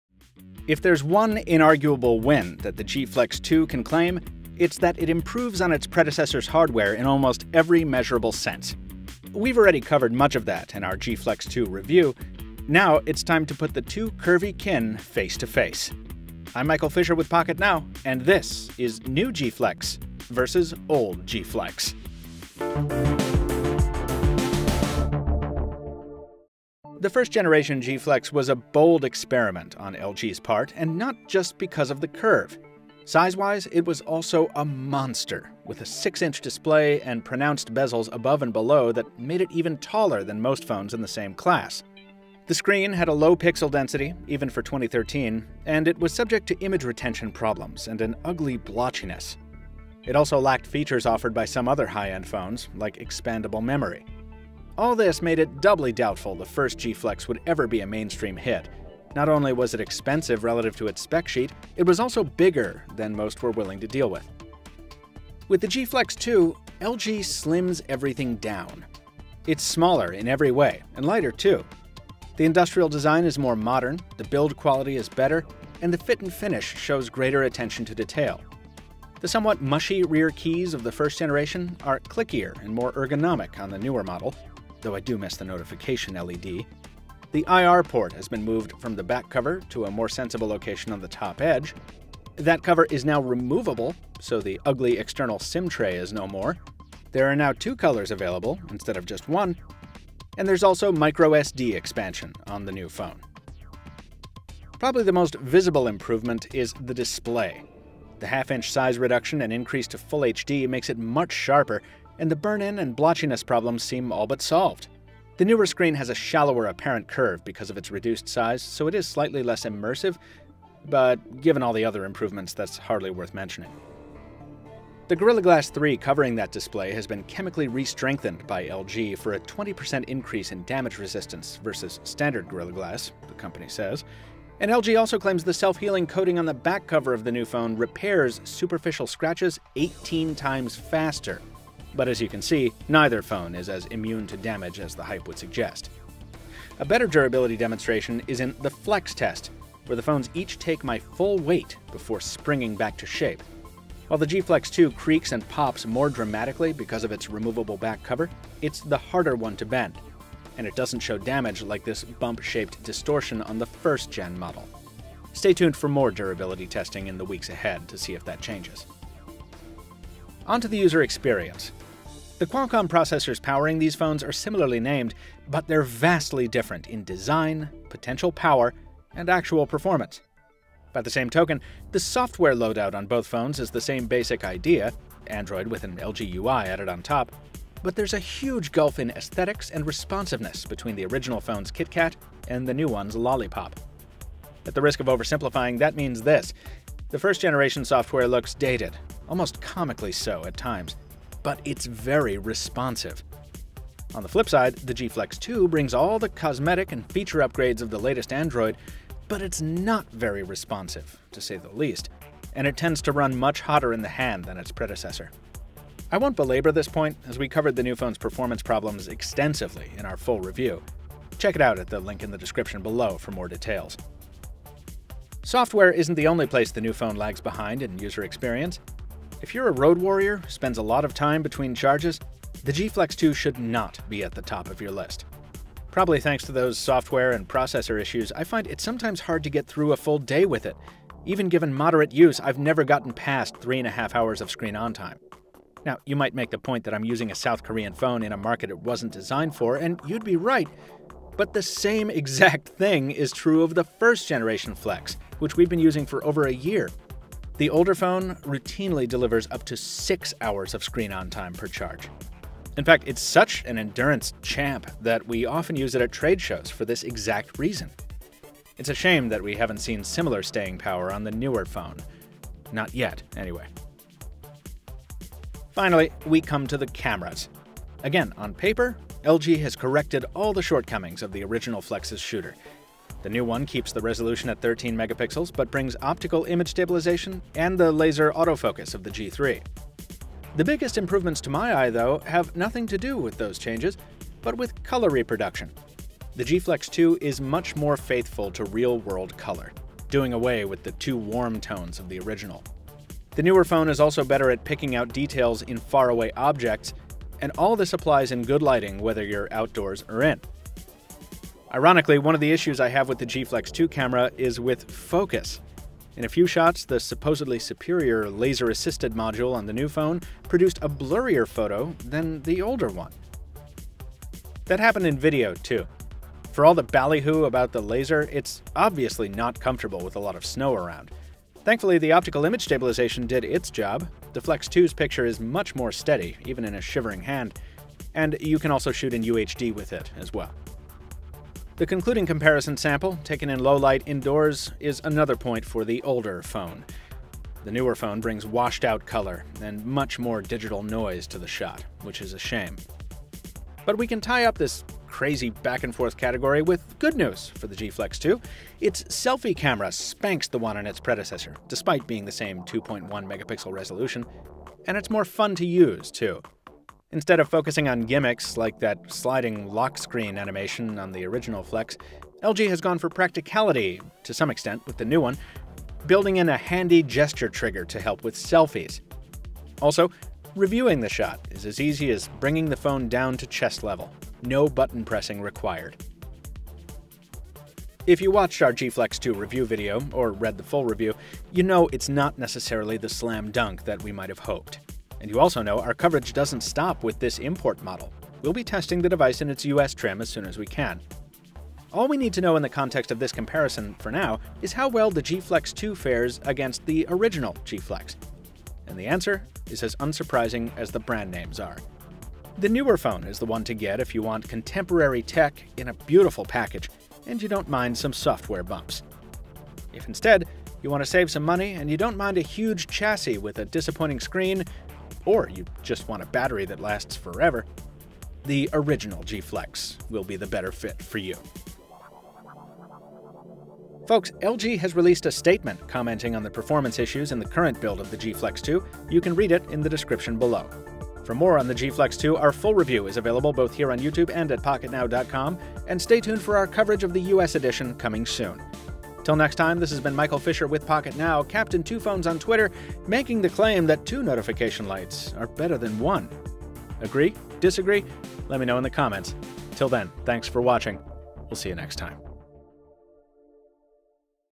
LG G Flex 2 vs LG G Flex | Pocketnow
For the full story on the LG G Flex 2, check out our in-depth review at Pocketnow: /2015/02/18/lg-g-flex-2-review

We called the first LG G Flex "the smartphone of the future," and our soft spot for it has endured ever since we reviewed it in 2013. As much as we enjoyed our time with it though, it was clear from day one that the curvy ergonomic conundrum was never destined to be a mainstream hit. It was expensive, it had a very lackluster display, and it was so big in the hand as to seem almost hilariously oversized at times.

With the G Flex 2, LG slims everything down. It’s smaller in every way, and lighter too. The industrial design is more modern, the build quality is better, and the fit and finish shows greater attention to detail. Most importantly, the display has been given a total overhaul: though it still retains its trademark 700R curve, it bears almost no sign of the ghosting and mottling problems of the previous generation. And those few details don't begin to scratch the surface of the LG G Flex 2 vs LG G Flex spec-besting story. In many ways, the G Flex 2 is what the first G Flex should have been to begin with.

But as you already know if you've read our G Flex 2 review, not everything newer is better in the land of curved smartphones. Between the smaller battery, iffy thermal performance, and sluggish Android build, there's plenty to give would-be upgraders pause. So which Flex should you consider when upgrade season kicks off this spring? Find out in our LG G Flex 2 vs LG G Flex comparison!

LG Statement on performance issues:
The devices sampled are representative of final industrial design and user experience but are continuing to undergo additional optimizations to enhance benchmark performance. We expect our upcoming software releases to provide further improvements in this area. We remain confident that the G Flex 2 will deliver great experiences to our customers with a tremendous blend of multimedia, performance and industry-leading design.